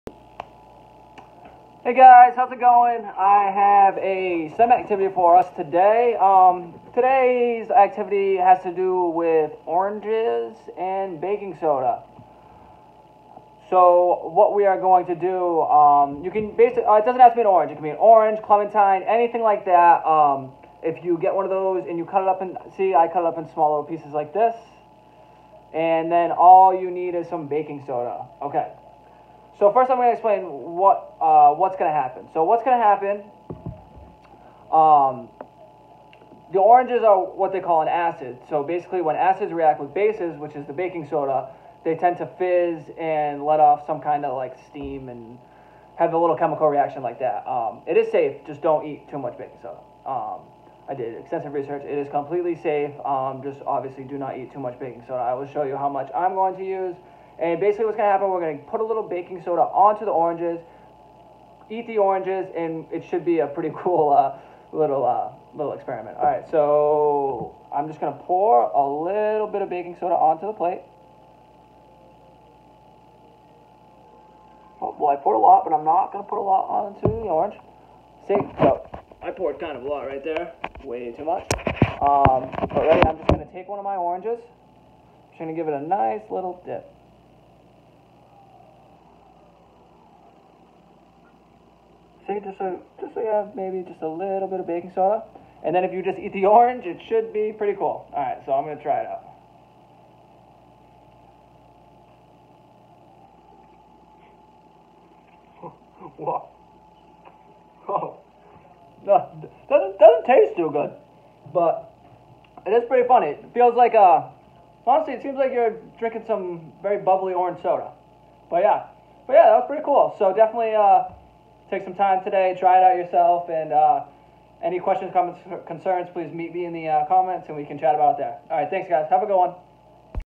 Orange Fizz
Hello Club Members! Today’s STEM activity is orange fizz, please take some time today to complete the activity.